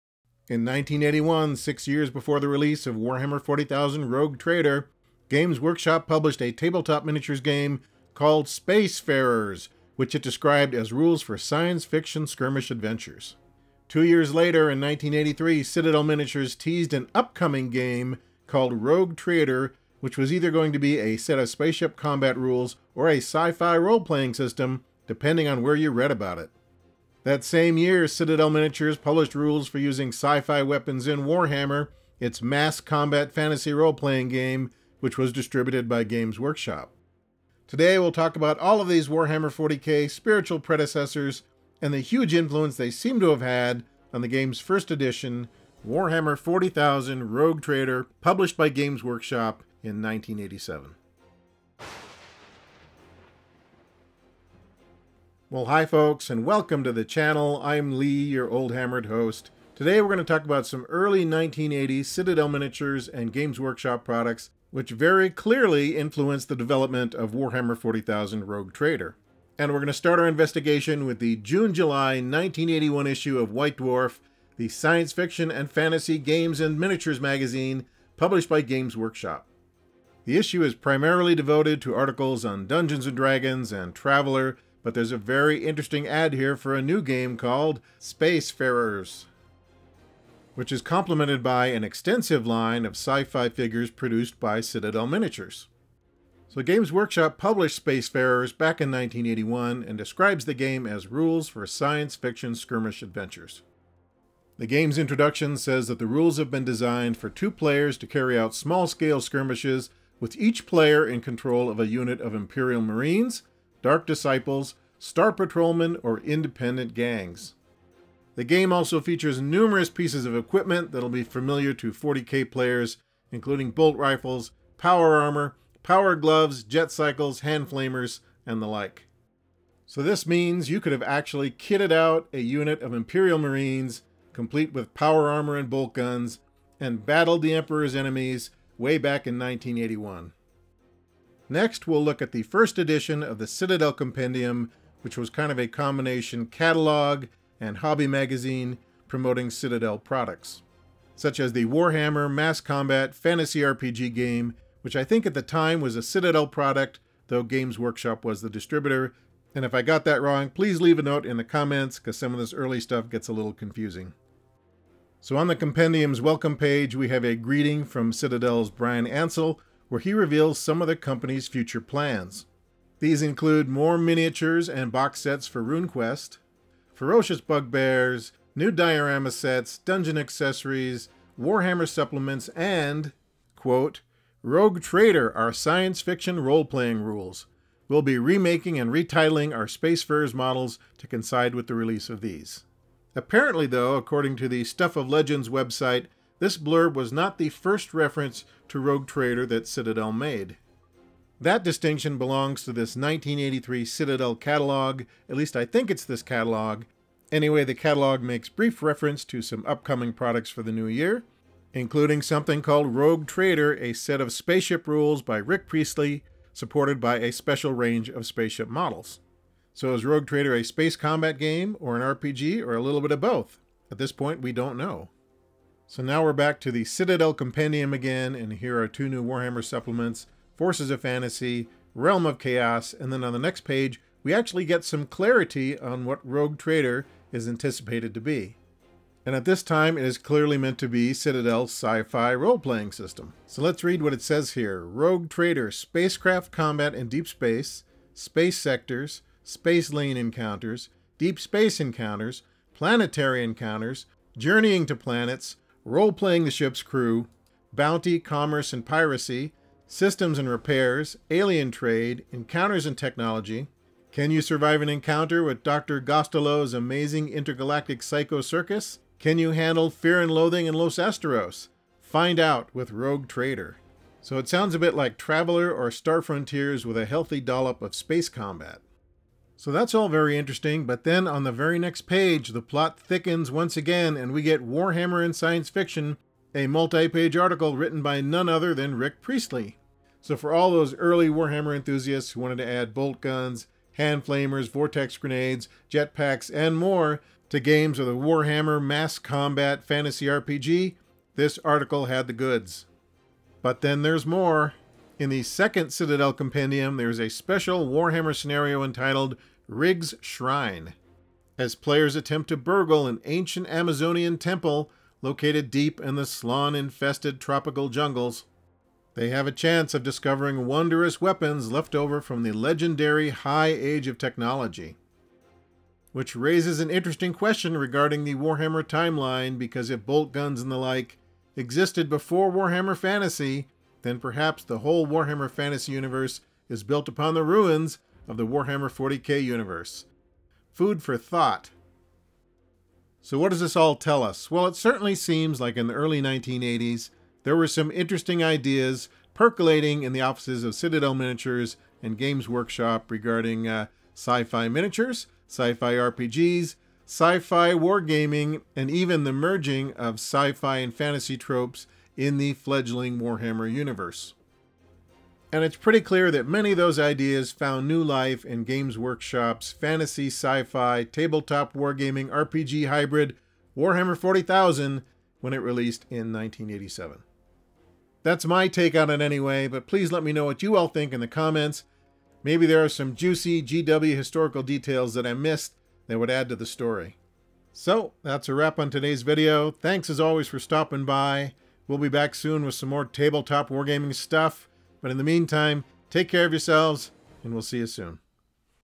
Warhammer 40k Ancestors: Spacefarers, Sci-Fi Warhammer and the Rogue Trader RPG
Join us on a trip back to the early 1980s as we explore early Games Workshop and Citadel Miniatures products that influenced the creation of Warhammer 40k.